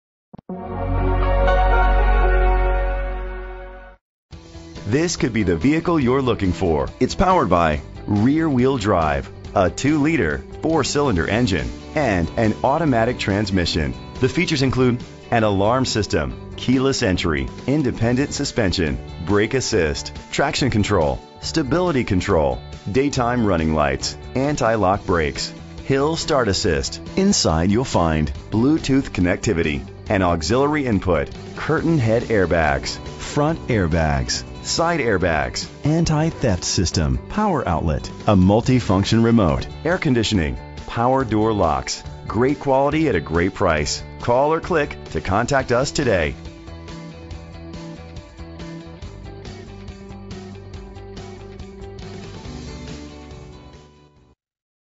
2016 Mercedes-Benz Metris M16318 - Union NJ
Phone:
Year: 2016
Make: Mercedes-Benz
Model: Metris
Trim: Cargo
Engine: 2.0 liter 4 Cylindercylinder RWD
Transmission: Shiftable Automatic
Color: Artic White
Mileage: 0
Address:
VIN:WD3PG2EA4G3097424